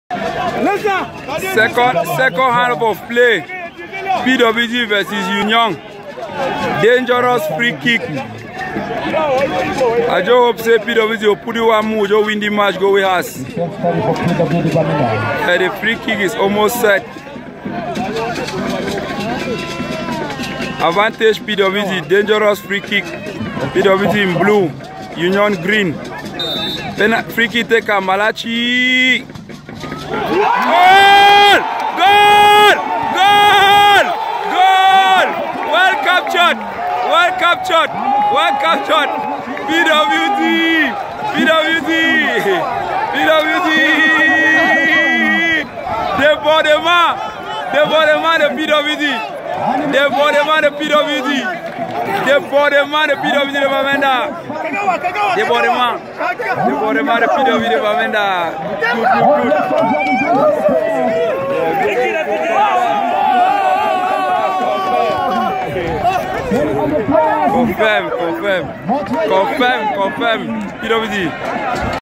PWD vs Union, 2018_2019 season. Great free kick from mundi Kagawa
A beautiful 30 yarder that gave pwd a comfortable 2#0 win